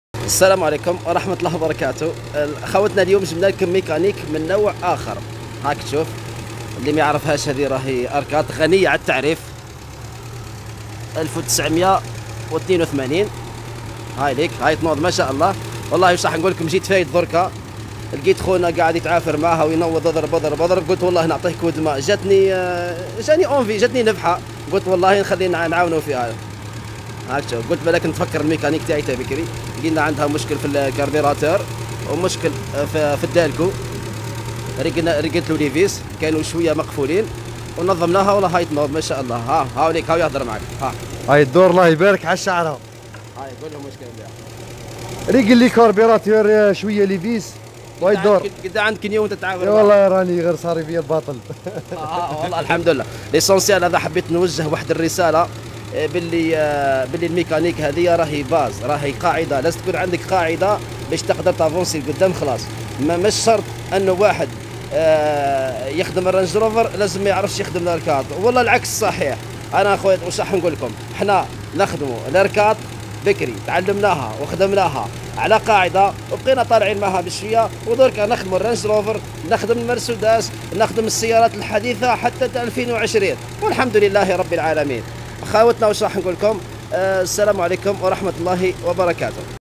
صاحب صفحة GME El Eulma يساعد شخص في سطيف لتصليح سيارة R4
بينما كان صاحب صفحة GME Groupe Mécanique El Eulma يتجول في مدينة سطيف صادف صاحب سيارة r4 يصلحها و منهمك .
جاه الفضول اساعده و الحمد لله تم التصليح في الشارع امام بيته ❤